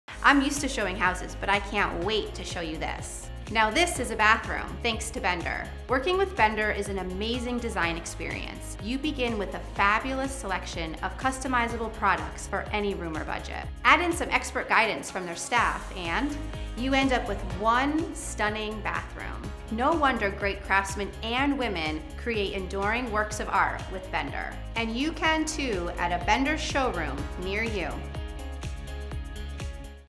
Nicole White Recommends Bender for Amazing Design
Work with Bender for an amazing design experience. Nicole White of The House That Social Media Built knows firsthand that with the help of design experts and a fabulous selection of customizable products, you'll end with the bathroom of your dreams.